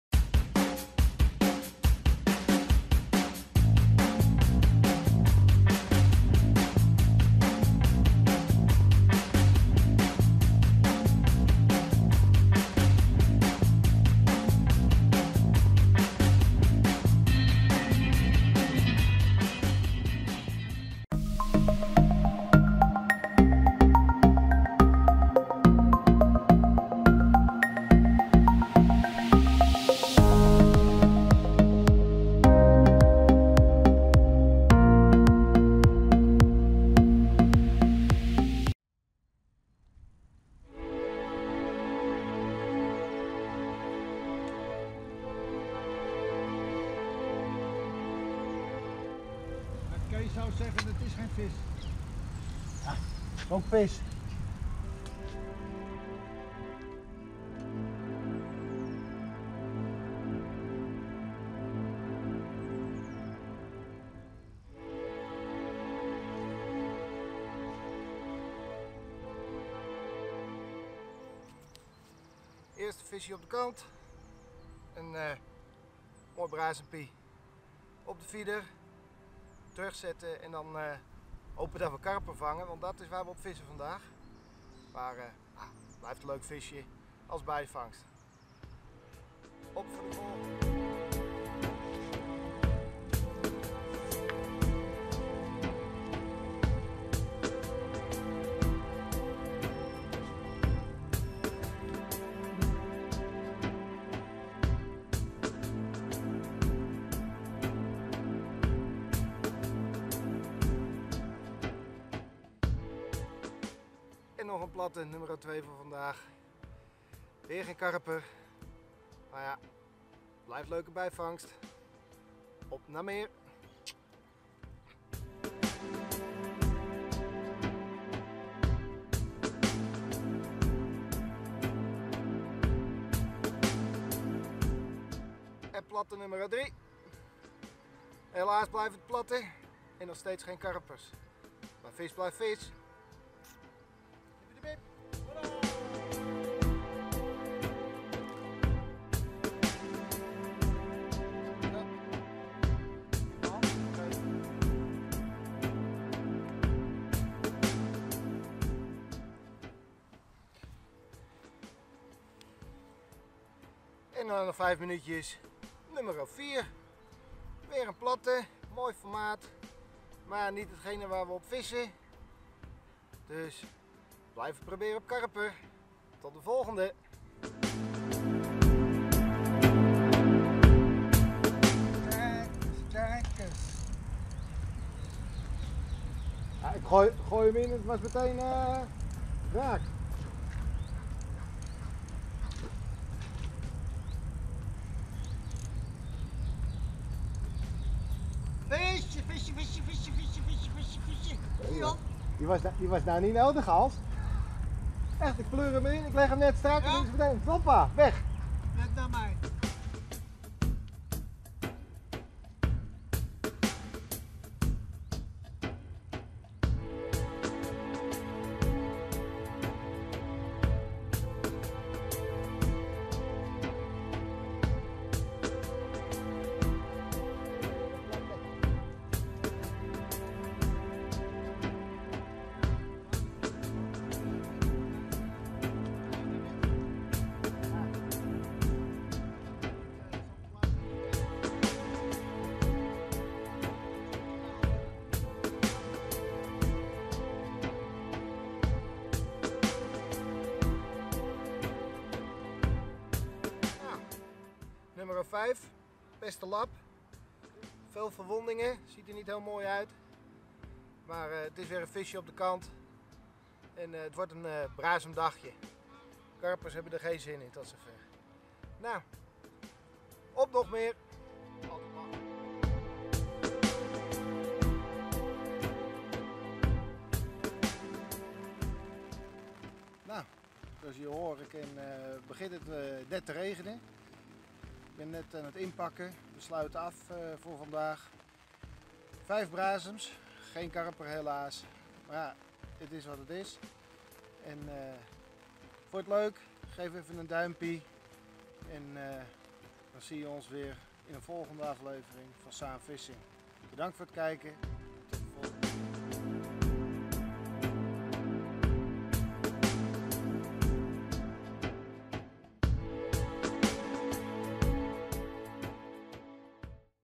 Feeder vissen -04- van karper naar brasem.
Het zou een dag worden om statisch te gaan vissen op karper, maar had voor het tijdverdrijf ook mijn feeder meegenomen.
De karpers hadden er geen zin in maar de brasems wel dus werd het een feeder filmpje.